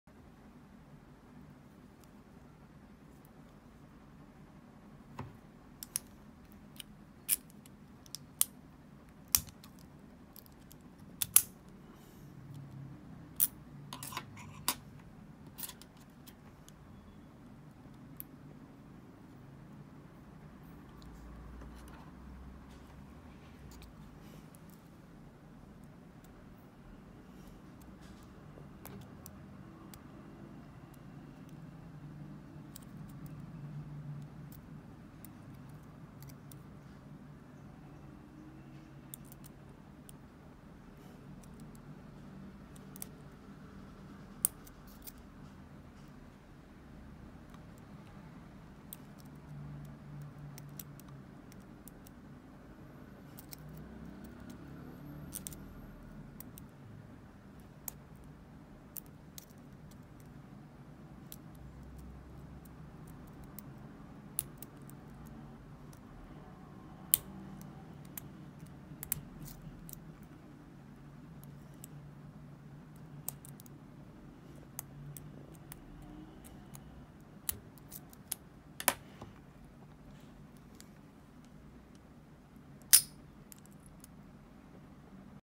[40] Picking Tri-Circle 265 Padlock
This padlock have hard spring on its lock. As you can see it takes me a force to turn the core to open the shackle.

It took me third attempt to be able to picked it open. Failed using bottom of keyway tension approach but work flawlessly with top of keyway tension.

It bent my stainless steel pick but mu spring steel pick able to force the pins to set. So I’m using Chinese made pick to defeat Chinese made padlock.

Hope you’ll enjoy my video 😁.